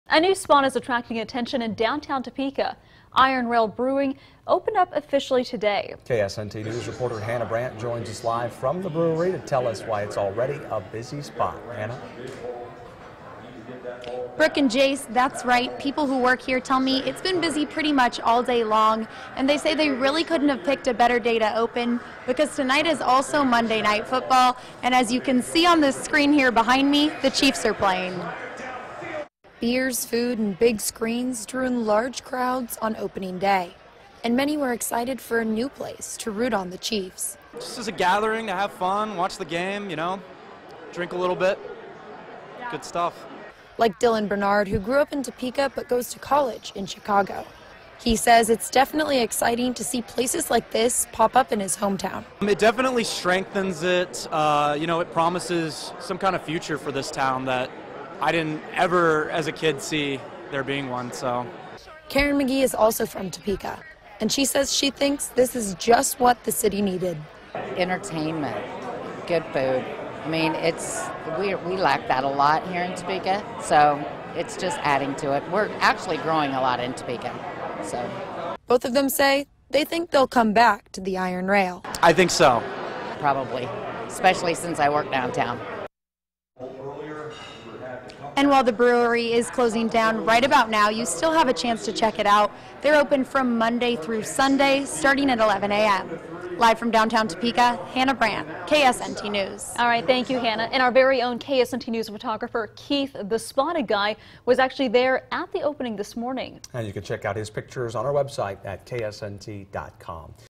Iron Rail Brewing brings in Chiefs fans on opening day
A new spot is attracting attention in downtown Topeka. Iron Rail Brewing opened up officially on Monday.